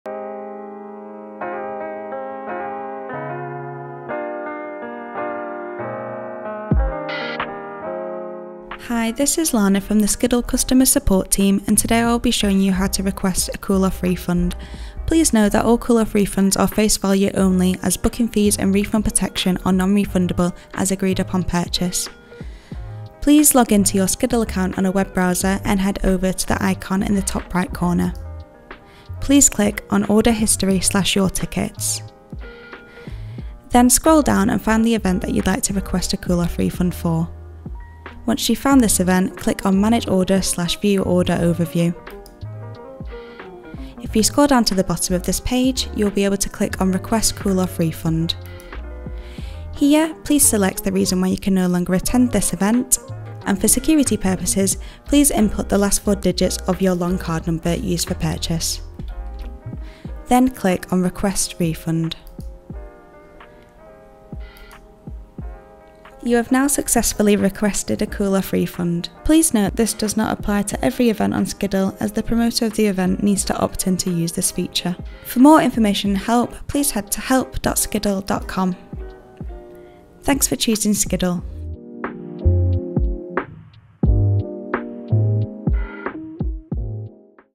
How To Request A Cool:Off Refund | Skiddle Customer Support
In 2016, we launched Cool:Off; the event industry's first refund window.

This offers you between 72 hours to 28 days (depending on the Cool:Off period the promoter chooses) window to claim a face-value refund if your circumstances change, with no questions asked.

Lannah from our Customer Support is on hand to show you how to do just that.

Please be aware, Cool:Off only applies to events where the promoter has opted in, therefore it does not apply for every event on Skiddle.

Cool:Off refunds are face-value. As agreed-upon purchase booking fees & refund protection are non-refundable.

Watch and Learn How To Request A Cool:Off Refund With Skiddle Customer Support.